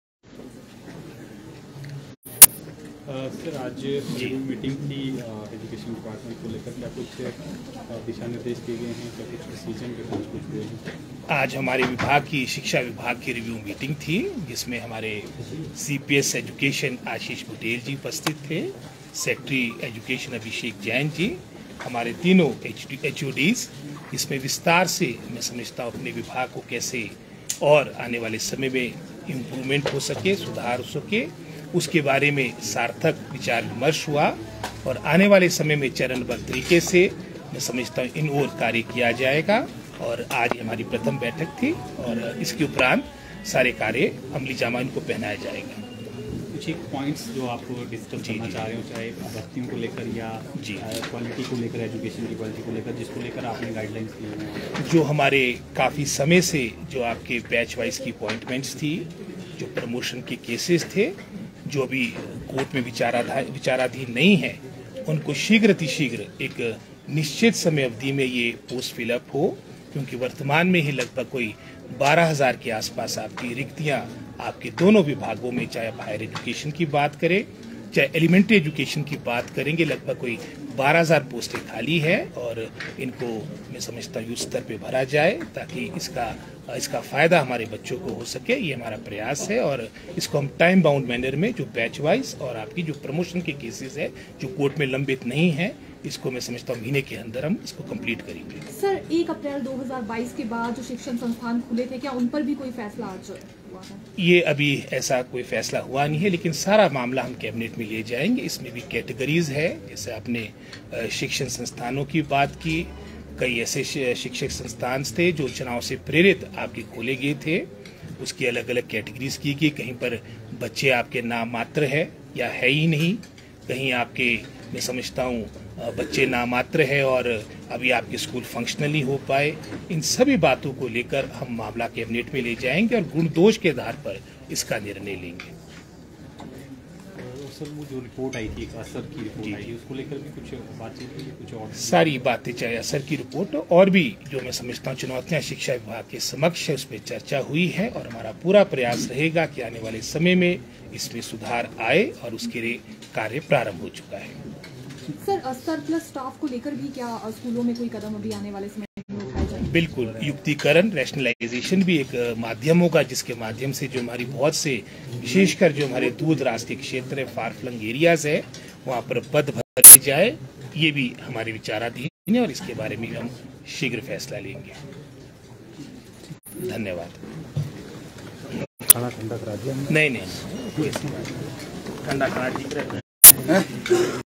शिक्षा विभाग की पहली समीक्षा बैठक समाप्त, क्या हुए निर्णय? दखिये क्या कहना है शिक्षा मंत्री रोहित...
शिक्षा विभाग की पहली समीक्षा बैठक समाप्त, क्या हुए निर्णय? दखिये क्या कहना है शिक्षा मंत्री रोहित ठाकुर का
First review meeting of Education Department ends, what were the decisions? See what Education Minister Rohit Thakur has to say